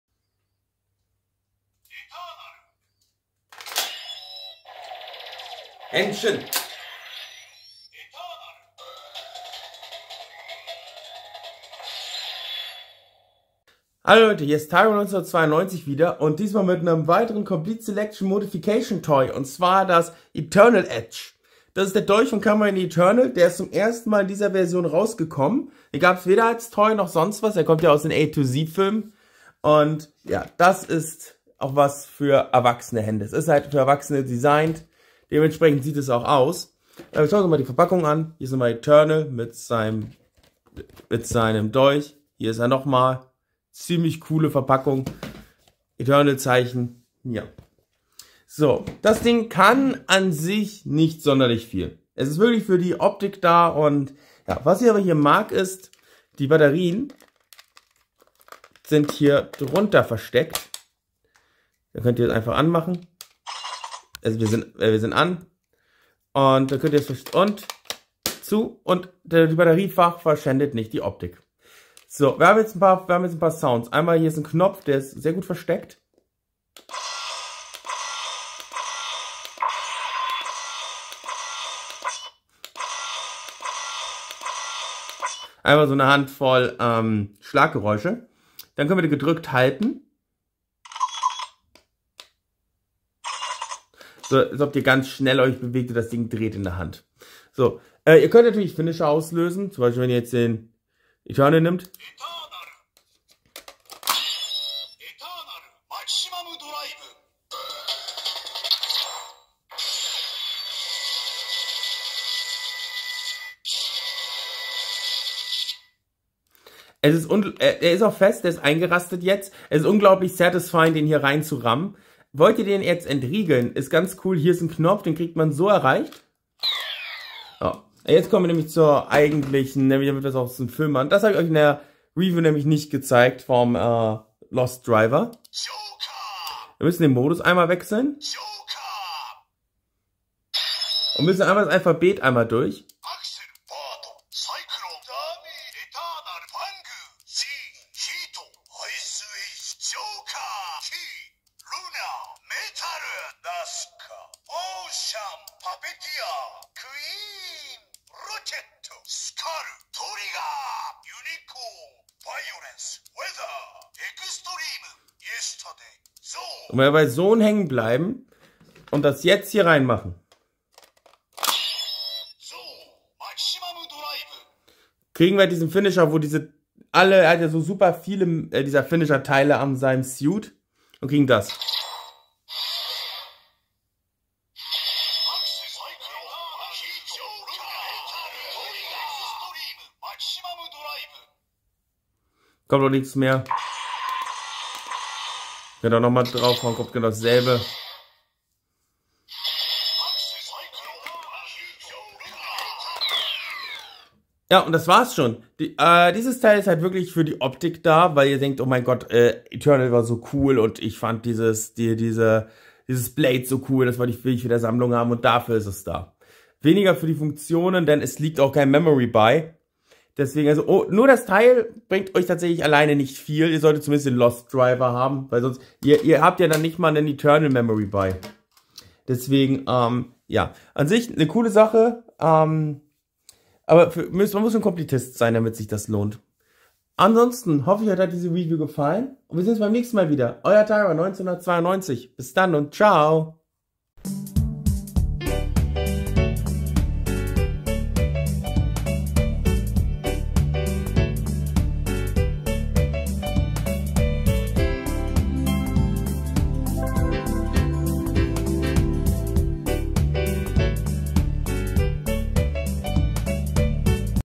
Kamen Rider W - CSM Eternaledge - Review
In diesem Video präsentiere ich euch das beeindruckende Eternaledge, die legendäre Waffe von Kamen Rider Eternal! Dieses Complete Selection Modification Produkt ist nicht nur ein absolutes Premium Toy, sondern auch perfekt an die Serie angepasst.

Begleitet mich auf dieser spannenden Reise, während wir das Eternaledge im Detail erkunden. Ich zeige euch die hochwertigen Materialien, die beeindruckenden Funktionen und die liebevollen Details, die dieses Sammlerstück zu einem Muss für jeden Kamen Rider-Fan machen.

Ob ihr bereits Fans seid oder einfach nur neugierig auf die Welt von Kamen Rider seit, dieses Video bietet euch einen umfassenden Einblick in die Faszination hinter dem Eternaledge.